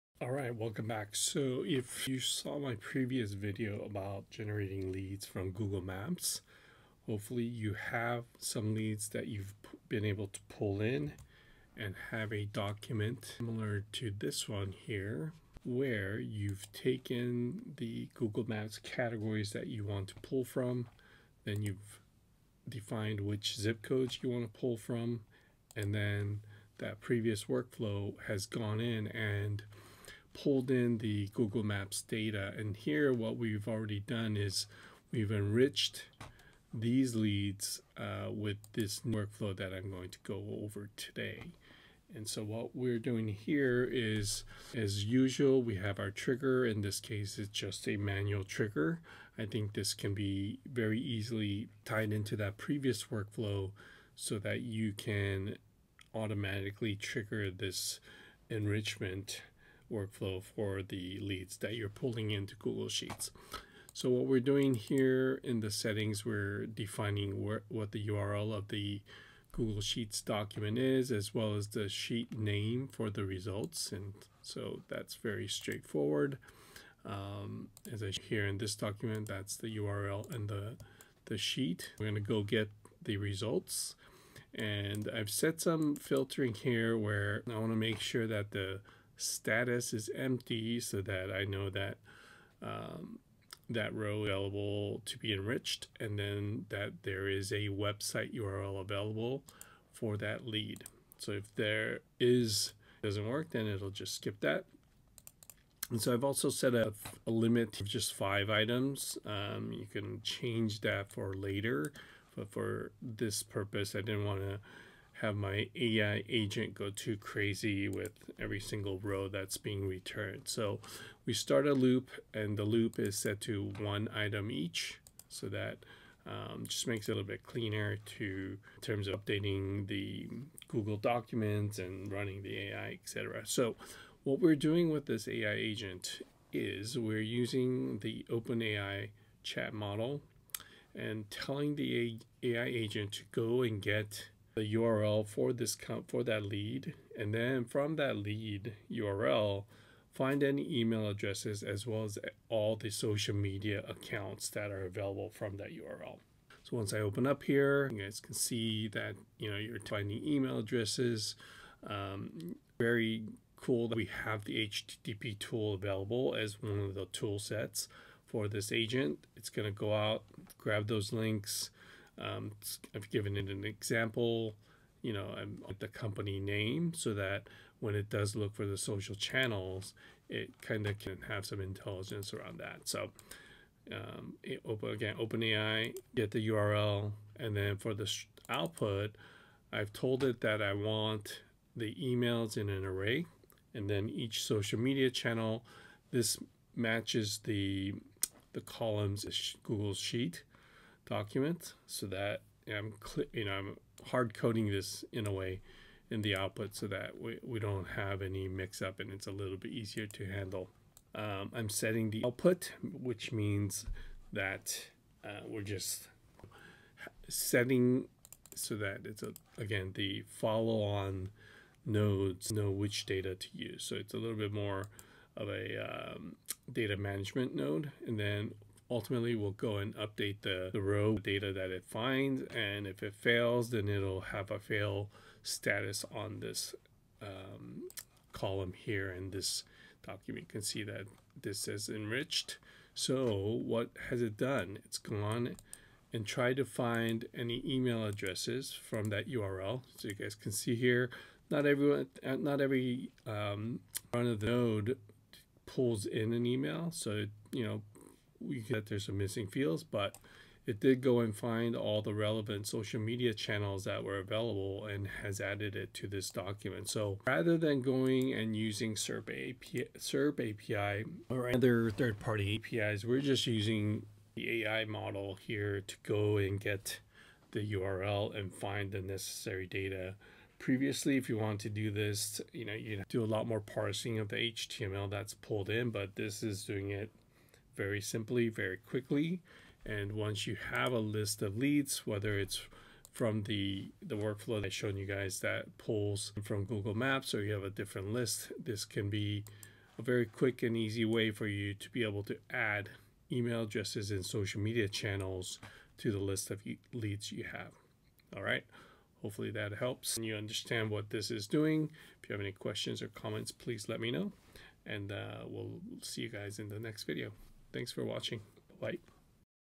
Find EMAILS and Social Media Links in SECONDS!
Discover how to find emails and social media links in seconds using n8n and AI agents. This lead generation tool is powered by Langchain technology for efficient and effective outreach.

*About Me:*
I’m Alex, an AI-Native Workflow Automation Architect building solutions to optimize your personal and professional life.